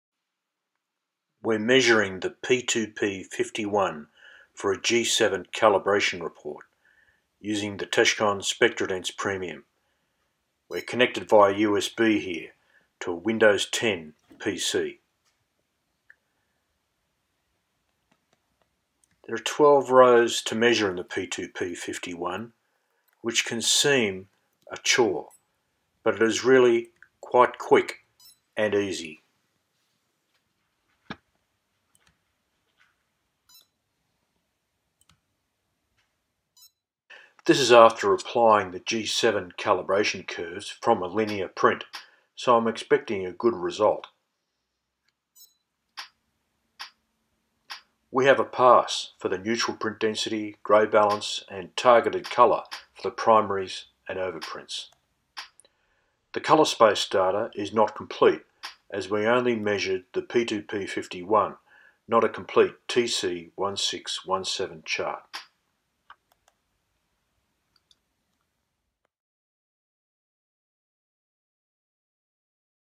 Measuring P2P51 Curve4 G7 Cal Report
Creating a G7 Calibration Report in Curve4 from measuring the P2P51 with the Techkon SpectroDens